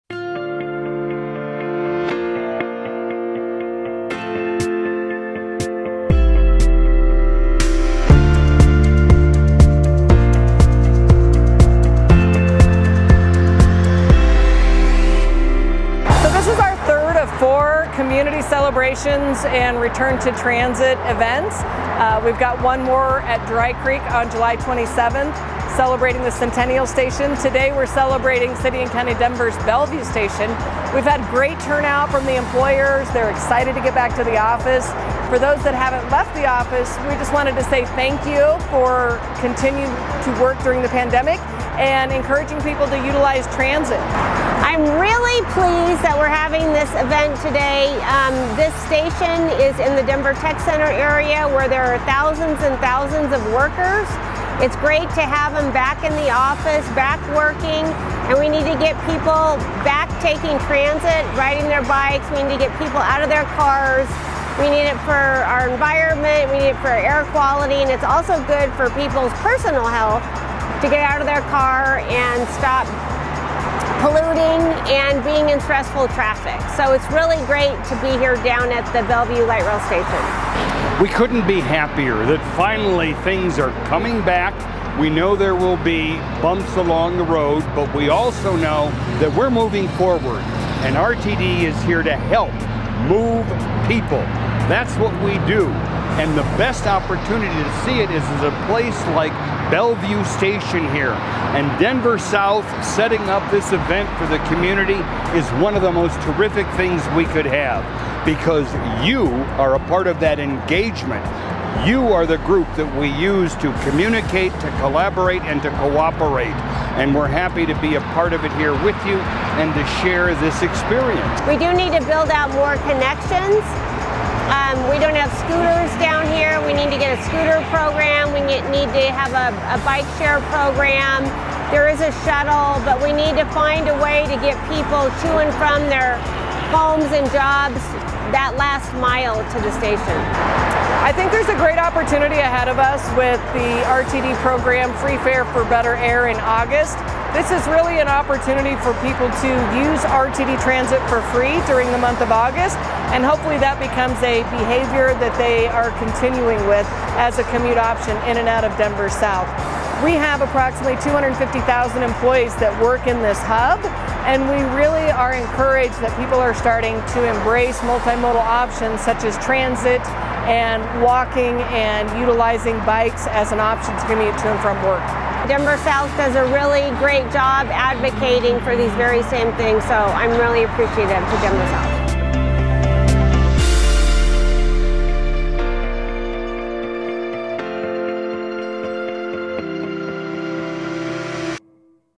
Denver South Community/RTD Celebration at Belleview Station
On July 13th Denver South, in conjunction with the City and County of Denver, hosted a Community/RTD Celebration at Belleview Station. This was our opportunity to thank our community for the tireless work they do in making Denver South a great place to live, work, and play! We hope you can join us for our final Community/RTD Celebration Wednesday, July 27th from 11 AM-1 PM at Dry Creek Station.